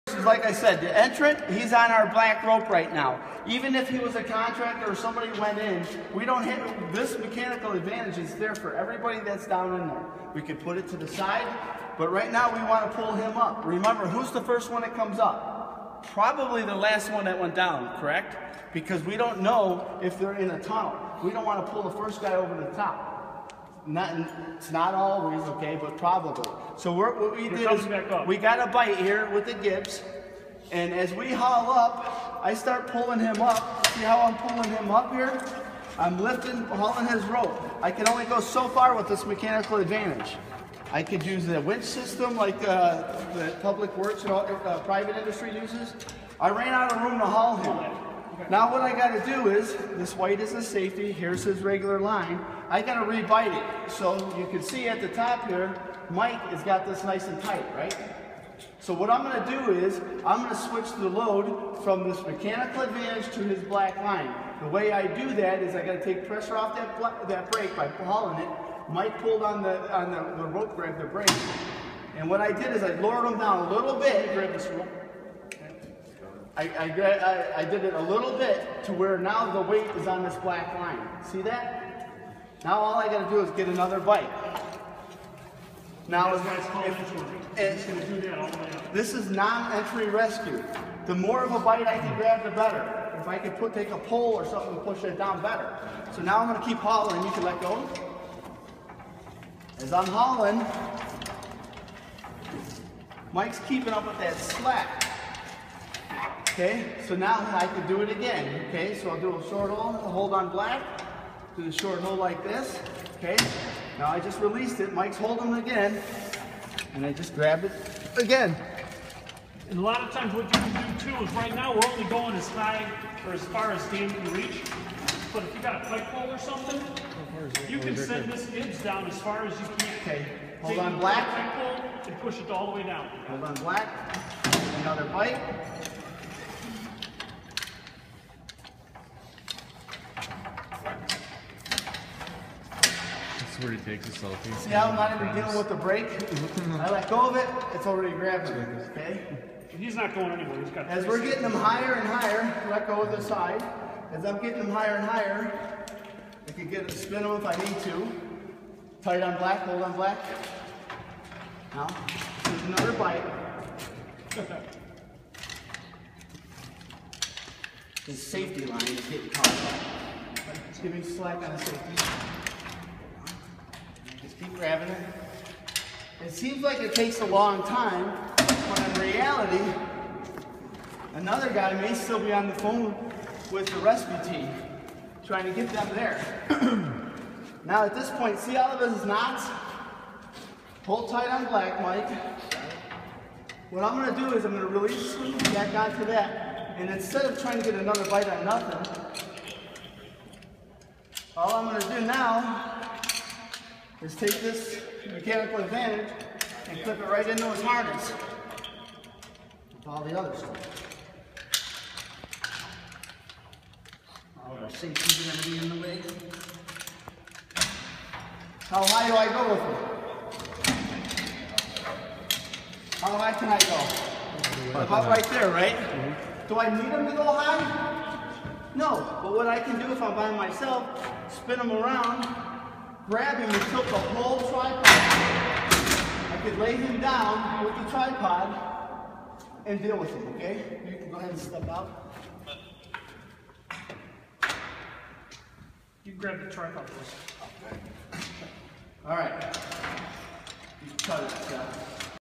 Permit-Required Confined Space Non-Entry Rescue
Non -Entry Rescue using a 4:1 M/A (Haulsafe)